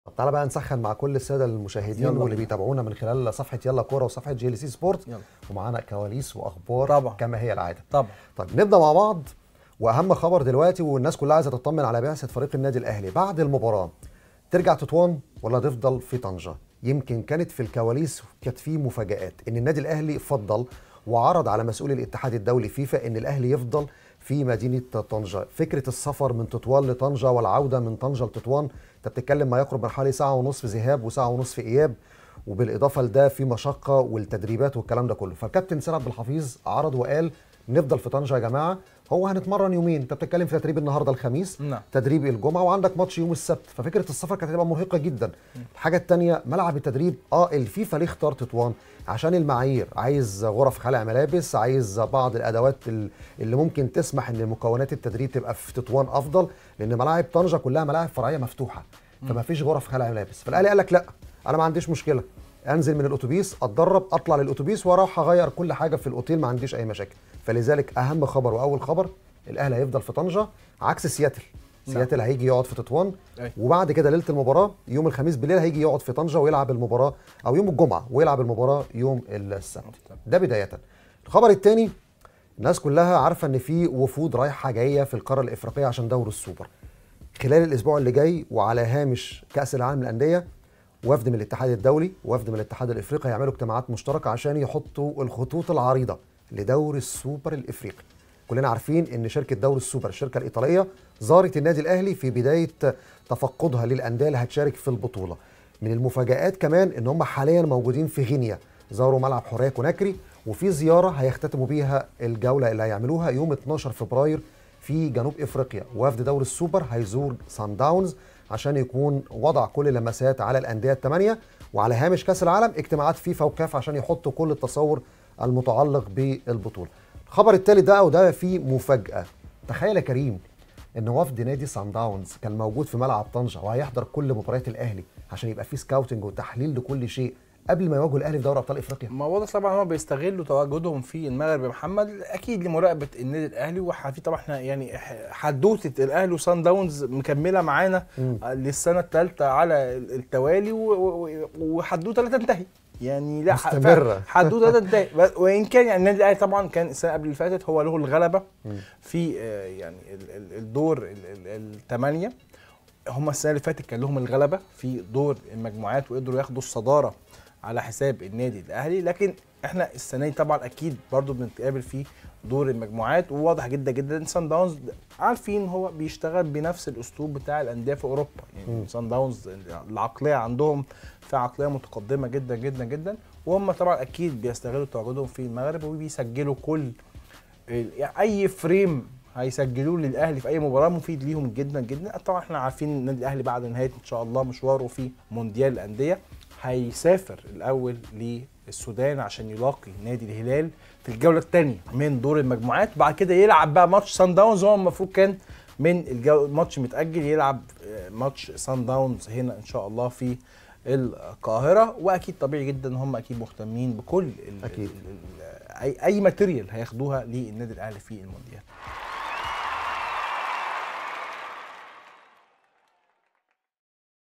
طلب من الأهلي لفيفا بعد الفوز على أوكلاند.. وصنداونز "يراقب" الأهلي في المونديال
طلب من الأهلي لفيفا بعد الفوز على أوكلاند.. وصنداونز "يراقب" الأهلي في المونديال.. أخبار حصرية مع محمد سعيد وكريم سعيد خلال برنامج